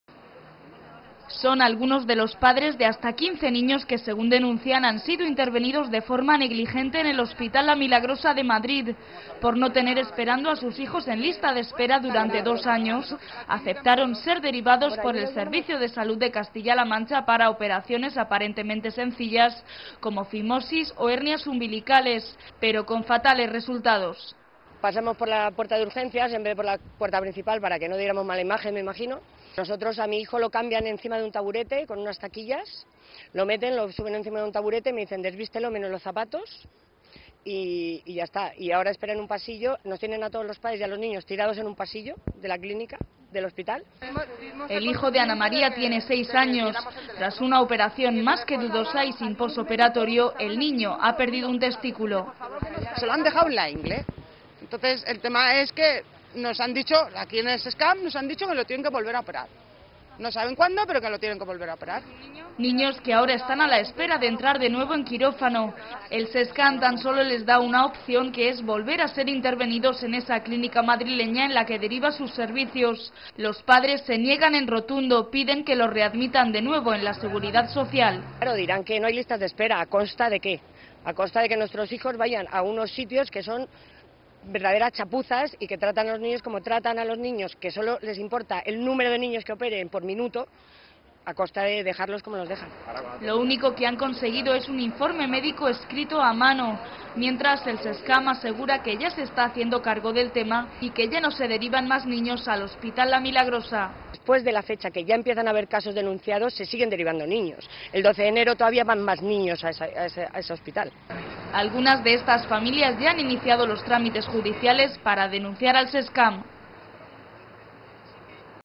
15 familias denuncian negligencias en operaciones a niños
Al menos 15 familias han denunciado que sus hijos han sido intervenidos de forma negligente en un hospital madrileño, derivados por el Sescam. Aseguran que el Servicio de Salud de Castilla-La Mancha presume de reducir listas de espera, pero no garantiza la buena praxis en los hospitales a los que deriva pacientes. Intervenciones, aparentemente sencillas, con resultados más que denunciables.